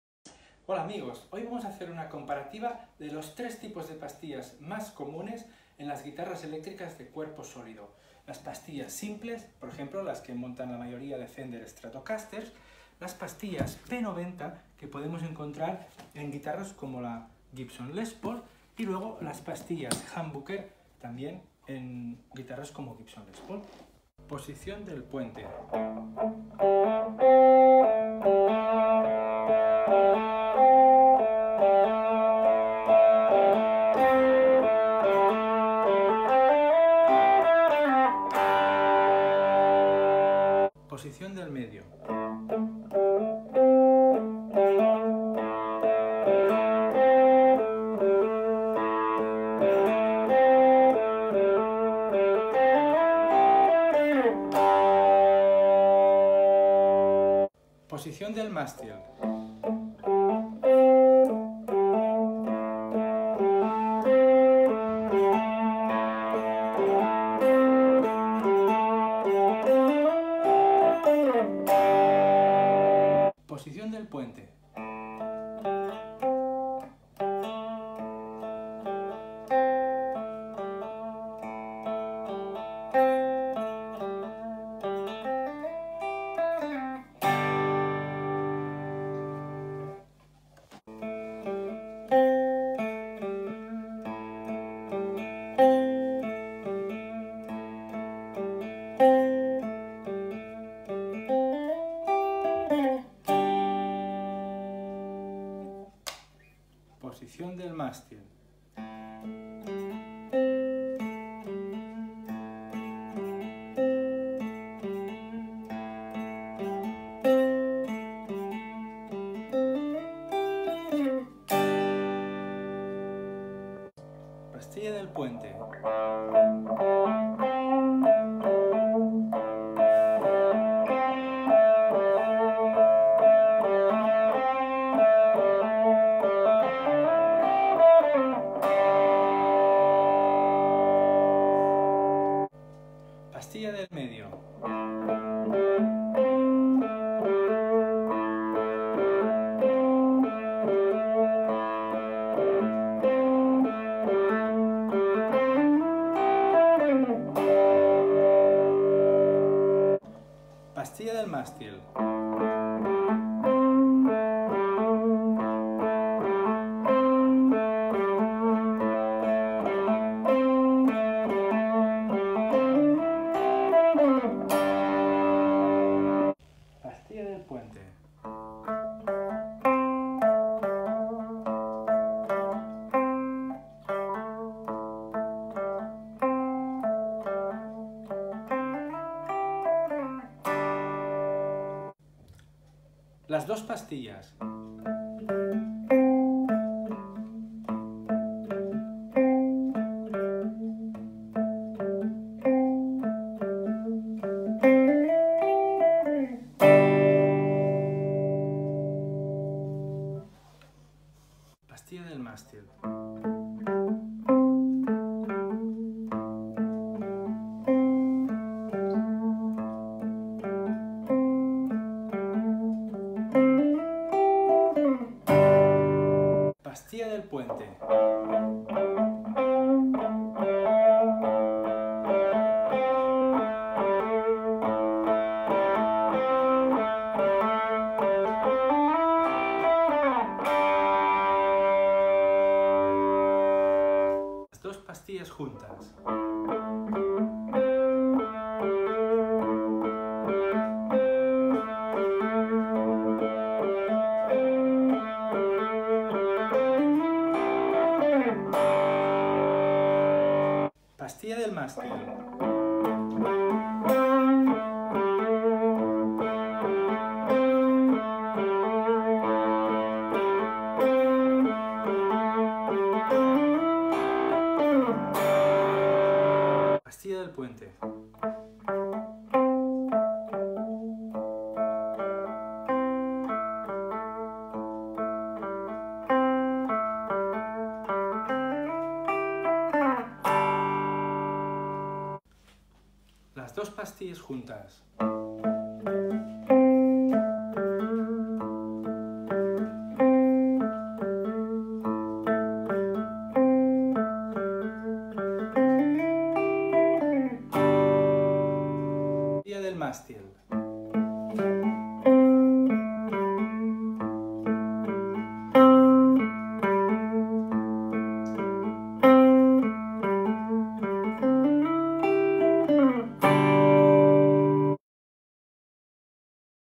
Pick up Comparision Comparativa Single coils,P90 & Hambuckers
Single Coil vs P90 vs Humbuckers Pickup Tone Comparison - Comparativa pastillas Simples, P90 & Hambuckers